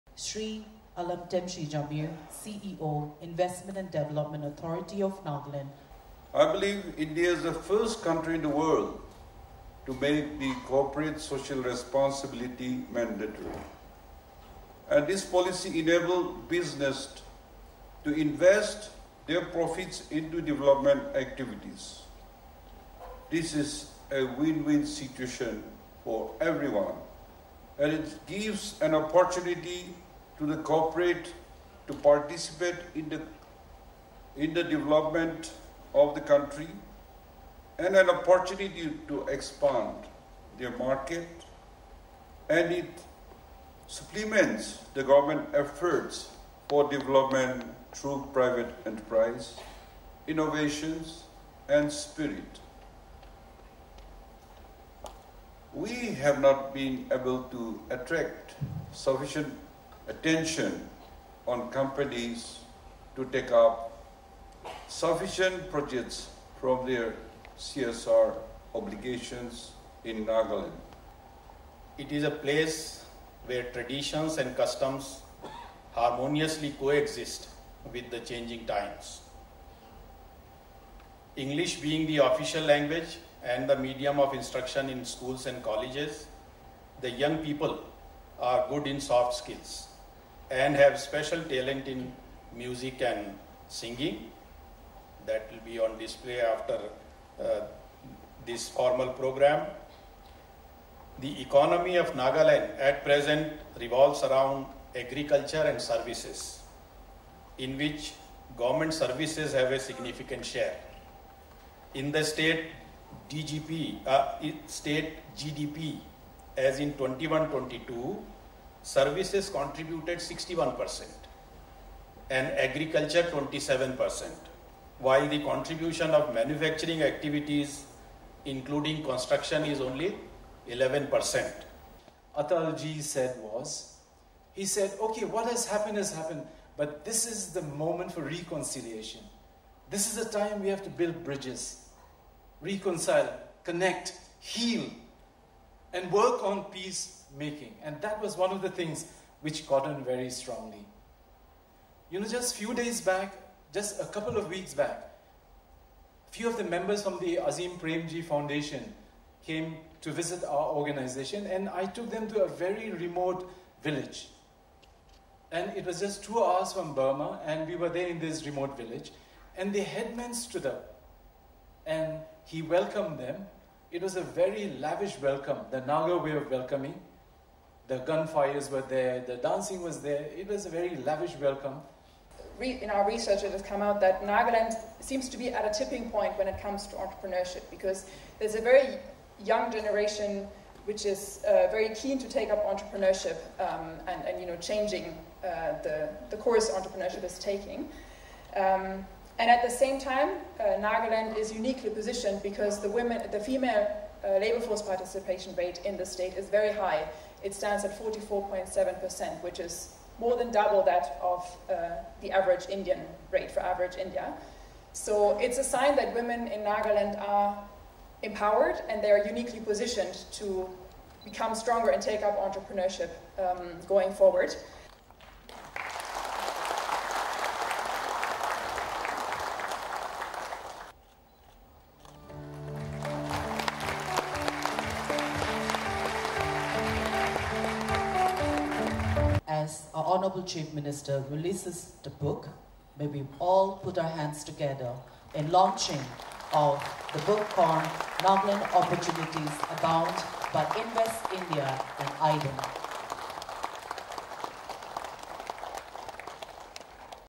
Precursor for the 1st Nagaland Corporate Social Responsibility Investment and Banking Conclave 2022
The precursor for the first Nagaland Corporate Social Responsibility CSR Investment and Banking Conclave 2022, was held last evening at Kohima. During the event, chief minister neiphiu Rio launched the First Nagaland Single Window System, and three apps of the Heath and Family Department, as well as the CSR project tracking and management system. He also disbursed an amount of nine point sixty seven crores to 738 goverment schools as first installment of Performace Incentive Grants under NECTAR- The Lighthouse. Speaking on the occasion, the Chief Minister said that India is the first country in the world to make Corporate Social Responsibility mandatory and this policy enables businesses to invest their profits in development activities. He said, this is a win-win for everyone as it gives an opportunity to corporates to participate in the development of the country and an opportunity to expand their markets, and it supplements the government efforts for development through private enterprise, innovation and spirit.  The  Chief Minister further stated that the Nagaland would like companies to be in the state for the long term, where they are able to integrate local businesses with the national supply chains. He added that CSR coupled with private investments would serve as an important supplement to Government spending, and maximize the impact of interventions in critical development sectors. Also addressing the programme, Chief Secretary of Nagaland, J Alam said, at present, Nagaland has sixteen administrative districts and is home to diverse indigenous tribes. Alam said, the present economy is mainly agriculture and government services that have a significant share. Founder of Eleutheros Christian Society, Reverend Doctor Chingmak Chang shared his experience of doing CSR in Nagaland. Project Head, Her and Now, Giz, Julia Karst,also addressed on the policy of women entrepreneurship in the state. Around sixty investors, bankers and CEOs from across the country are taking part in the conclave.